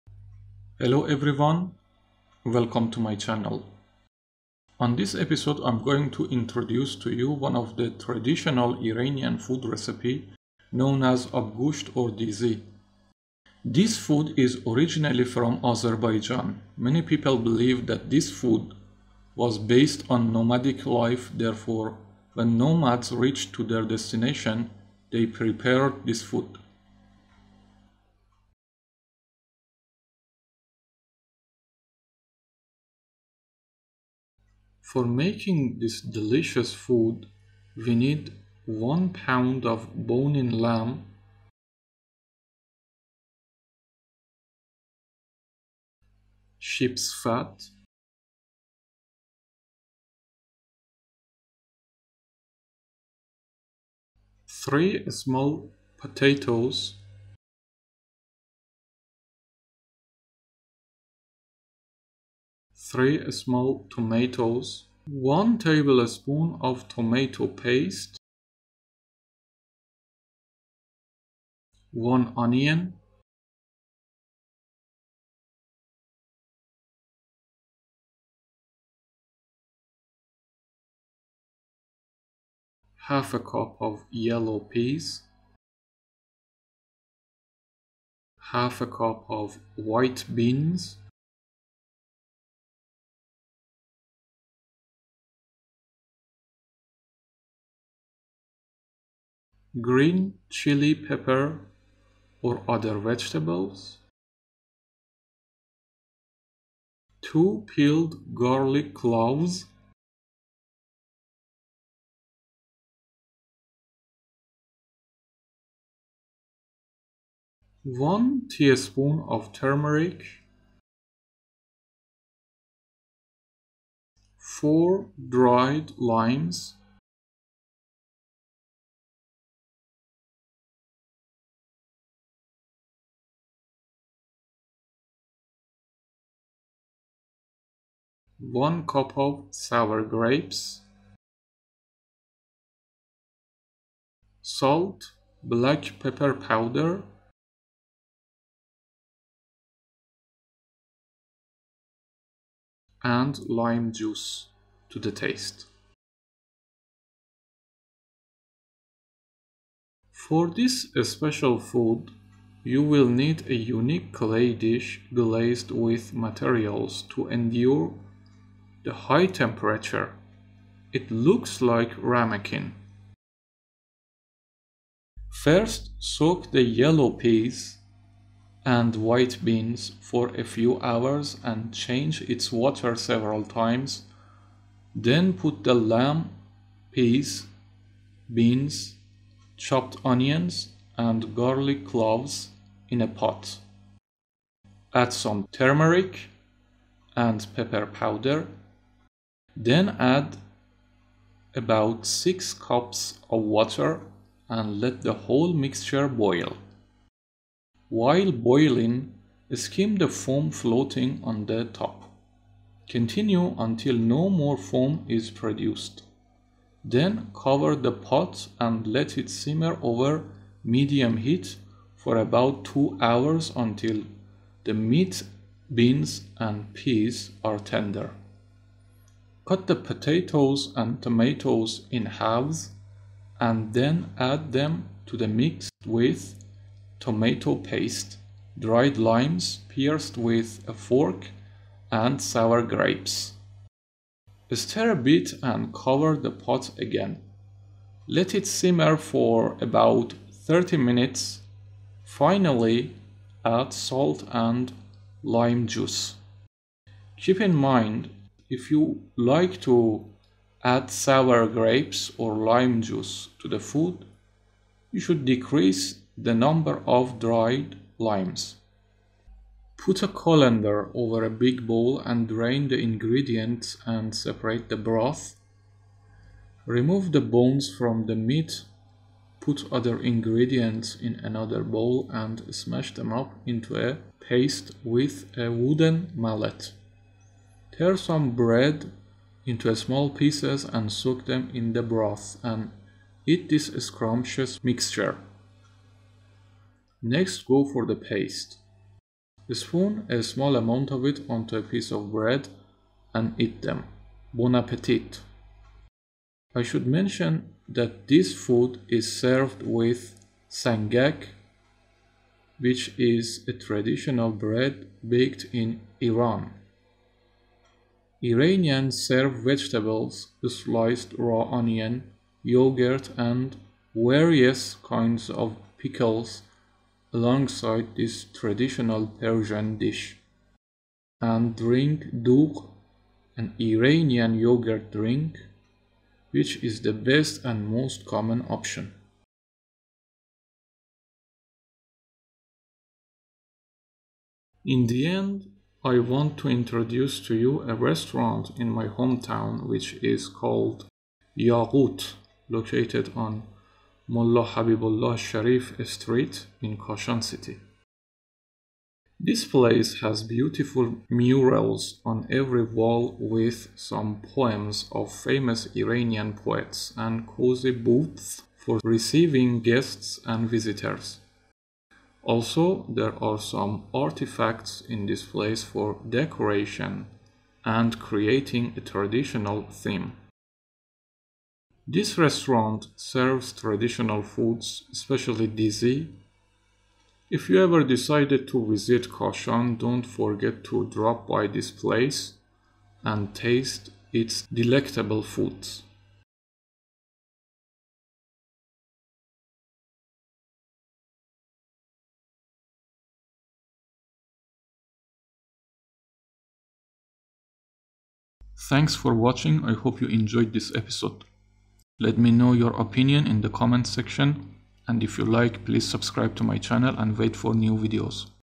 Persian food recipe (Dizi or Abgoosht)
Dizi or Abgoosht is one of the most traditional Iranian food
take a look at the Yaghoot restaurant at the end of this video!
Yaghoot restaurant located on Mollah Habibollah Sharif Street in Kashan city
 دستور پخت دیزی بهمراه زیرنویس فارسی ودیزی سرای یاقوت واقع در خیابان ملاحبیب الله شریف-شهرکاشان

00:00 Introduction
00:30 Ingredients
02:55 Directions to Cook
04:29 How to serve
05:40 Yaghoot restaurant